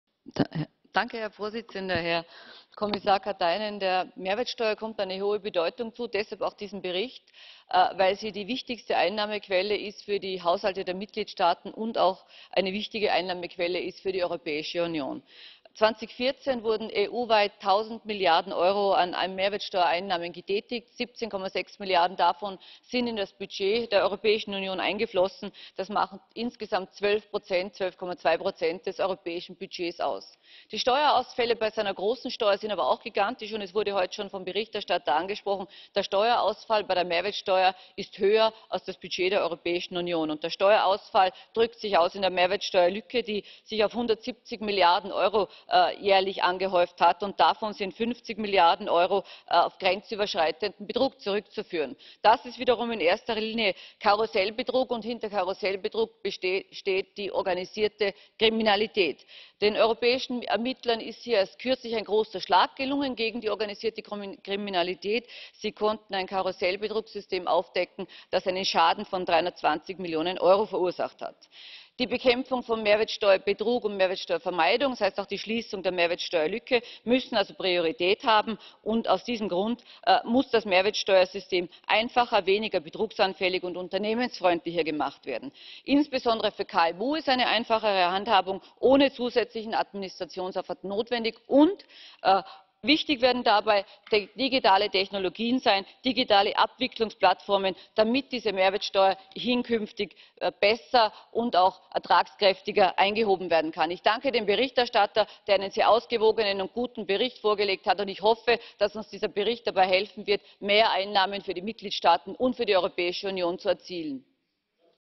Barbara Kappel - Mehrwertsteuersystem und Betrugsprävention
Plenartagung im Europäischen Parlament vom 23/11/2016, Thema: Wege zu einem endgültigen Mehrwertsteuersystem und zur Bekämpfung von Mehrwertsteuerbetrug (Aussprache)